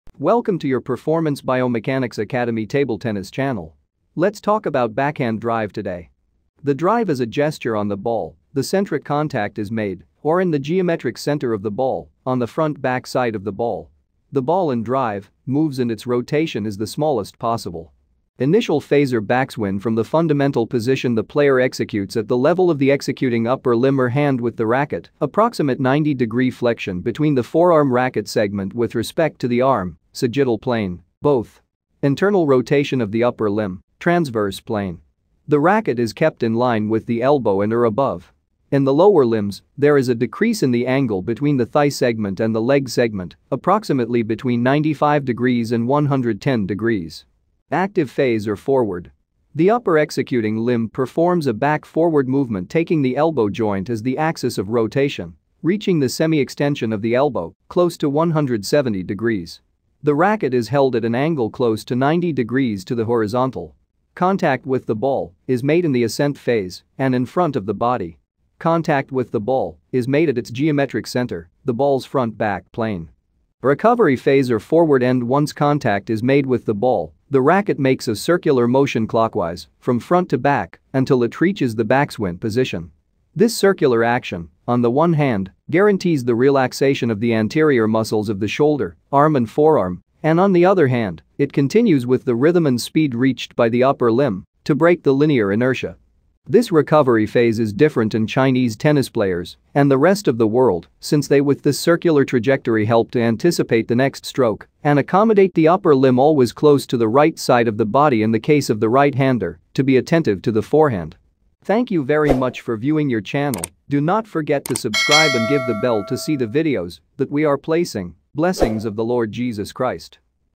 Fan Zhendong. How to do backhand drive in table tennis. This is for correction.
How to do backhand drive in table tennis. This is for correction.

The backhand drive in table tennis is a stroke and gesture on the ball classified in precision, its objective is to transfer the ball with the back of the hand racket, and is the basis of the block.
In this video we will observe details of the execution of Chinese table tennis players, which distinguish them from the rest of the players in the world.

Preparatory phase

Starting from a basic position, the upper executing limb (MSE) performs an internal rotation (shoulder adduction), flexing at the elbow joint and rotating the forearm, leaving the palm of the hand upwards (supination) forming an angle. 90 ° between racket-hand, elbow and shoulder
Contact or active action phase

- The MSE performs a back-forward movement taking the elbow joint as the axis of rotation; reaching the semi-extension of the elbow. (-180 degrees).
- The racket is held at an angle close to 90 degrees to the horizontal.
- contact with the ball is made in the ascent phase and in front of the body.

Final movements phase

- The racket culminates at the level of the face in front of it, to place the body in position for a later execution.

The backhand drive in table tennis is an offensive shot played close to the table that requires good timing, technique, and control. Here's a step-by-step guide for correcting and improving your backhand drive:

**1. Stance and Positioning:**
- Stand with your feet shoulder-width apart.
- Position yourself close to the table, but not too close that you feel cramped.
- Keep your knees slightly bent to maintain balance and agility.

**2. Grip:**
- Hold the racket with a backhand grip. Your thumb should rest against the rubber side, and your fingers should wrap comfortably around the handle.

**3. Ready Position:**
- Hold the racket in front of you with the racket head pointing slightly upward.
- Your non-playing hand should be used for balance and to help guide your shot.

**4. Preparation:**
- As the ball approaches, pivot your body slightly and rotate your hips to the side to create space for your swing.
- Your racket hand should move back and slightly down, preparing for the stroke.

- Move your racket forward with a controlled motion as the ball reaches the desired contact point.
- Make contact with the ball on the front side of your racket.
- Aim to hit the ball at the peak of its bounce or slightly after.

**6. Follow-Through:**
- After making contact, extend your arm and wrist smoothly and naturally.
- Allow your racket to follow through across your body, pointing toward the opposite side of the table.

**Common Corrections:**
1. **Timing:** Ensure that you're meeting the ball at the right moment, ideally just as it reaches its highest point after the bounce. Late timing can lead to errors.

2. **Racket Angle:** Maintain the correct racket angle for the desired shot. For a backhand drive, the racket should be nearly parallel to the table's surface.

3. **Body Rotation:** Rotate your hips and torso to generate power and engage your core muscles in the shot. A lack of rotation can result in a weaker shot.

4. **Wrist Flexibility:** Allow your wrist to be flexible, especially during the contact phase. This flexibility enables you to generate spin and control on the ball.

5. **Follow-Through:** A smooth and controlled follow-through is crucial for accuracy and control. Make sure your racket completes its motion across your body.

6. **Footwork:** Position yourself appropriately for the shot. Being too close or too far from the table can affect your ability to execute the backhand drive effectively.

7. **Practice and Patience:** Correcting and refining your backhand drive requires consistent practice. Focus on one correction at a time and gradually integrate improvements into your overall technique.

Remember that mastery of the backhand drive takes time and patience. Regular practice, along with paying attention to these key elements, will help you improve your backhand drive and become a more well-rounded table tennis player. If possible, seek feedback from a coach or experienced player to aid your correction process.